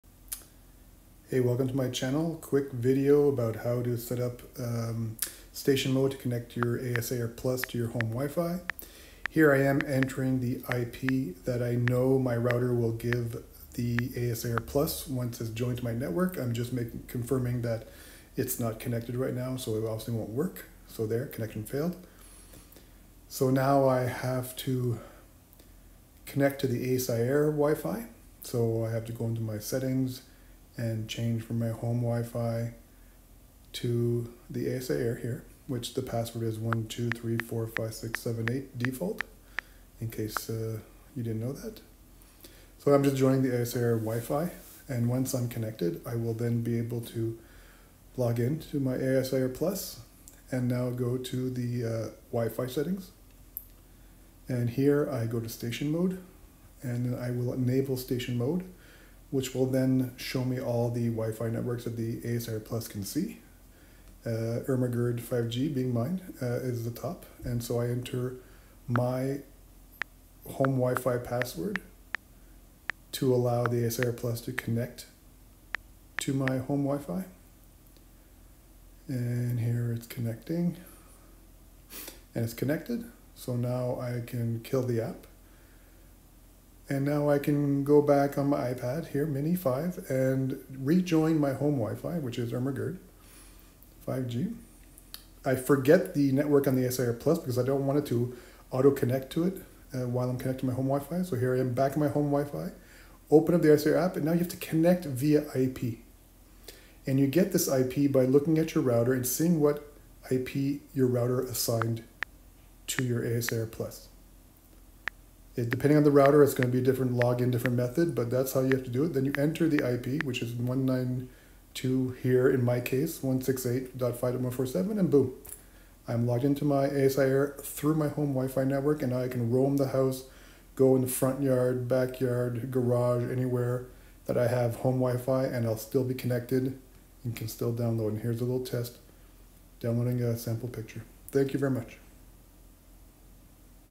ASIAIR Plus - STATION MODE
A quick video explaining how to set up station mode on your ASIAIR plus to have a connect to your home Wi-Fi at 2.4 GHz Wi-Fi speeds